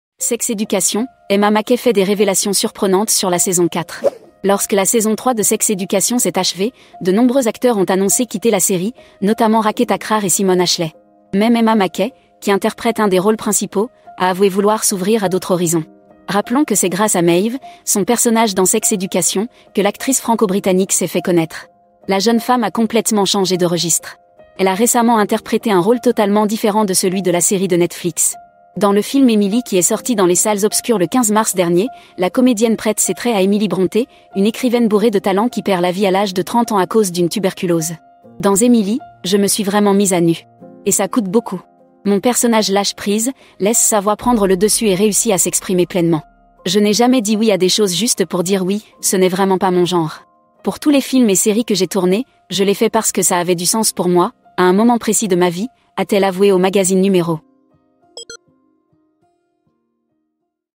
Sex Education : Emma Mackey fait des révélations surprenantes sur la saison 4 !
Emma Mackey fait certaines révélations sur la saison 4 de Sex Education aux fans !